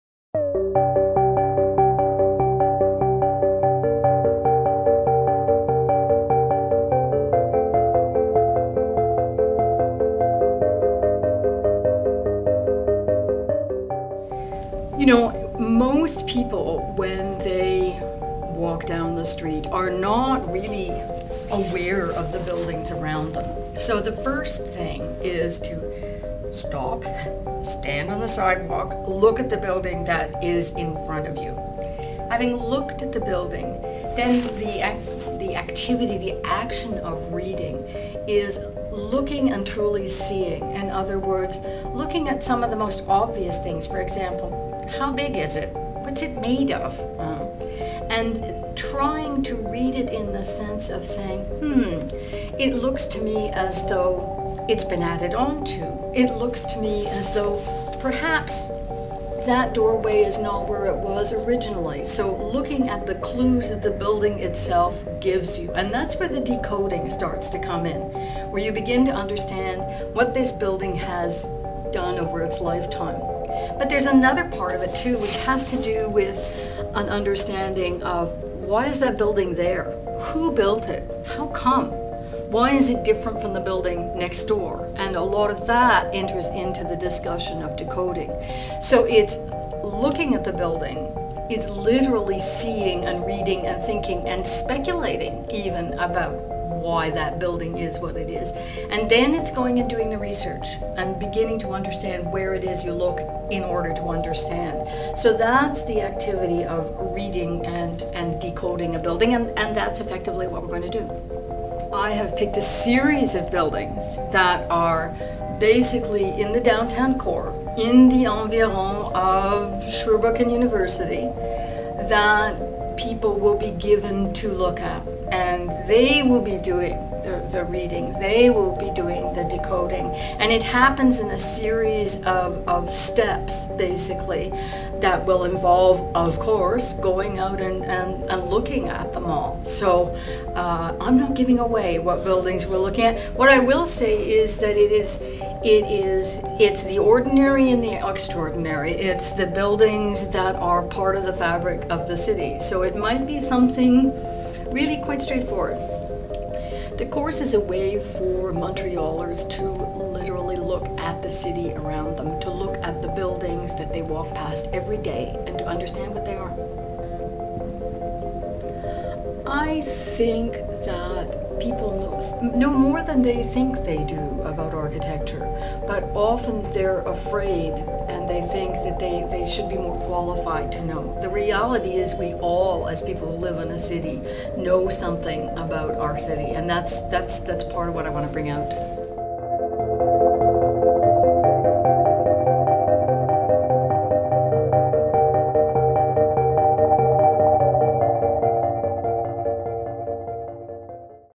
Nancy Dunton - The Buildings All Around Us - Part 2
McGill School of Continuing Studies' One Day @ McGill presenter Nancy Dunton talks about being aware of our architectural space and what it means to read and decode a building. For more information on her upcoming course, The Buildings All Around Us,